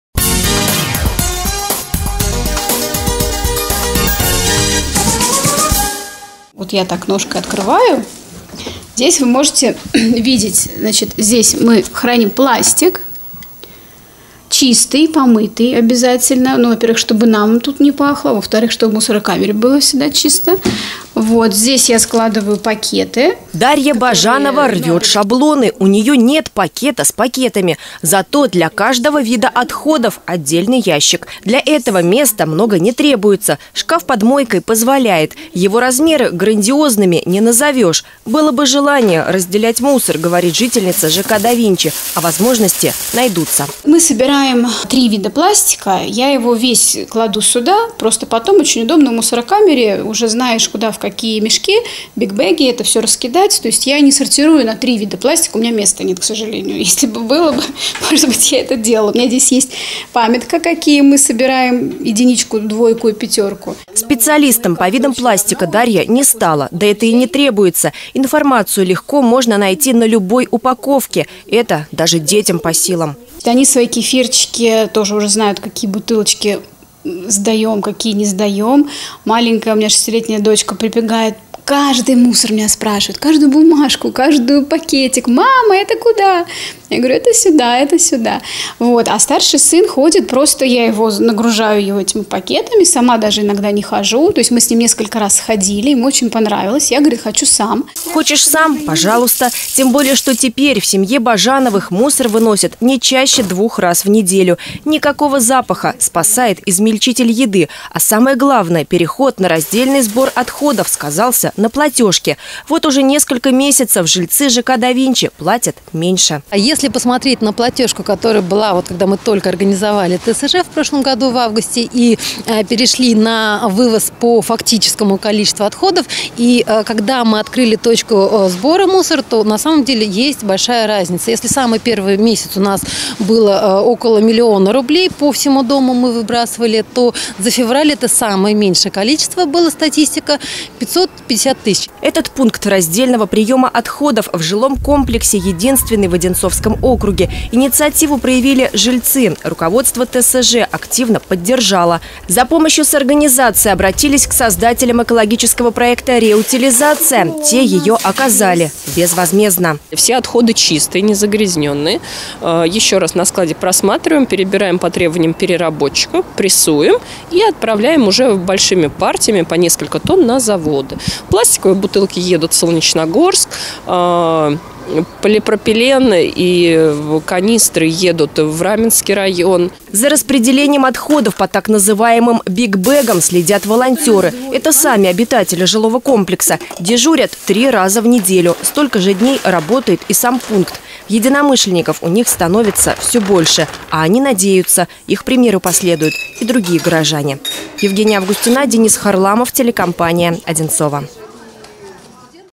Раздельный сбор
В жилом комплексе «Да Винчи» работает пункт приема вторсырья. Решение ответственно утилизировать отходы приняли жители, руководство товарищества собственников жилья их активно в этом поддержало. Мы узнали, как работает система.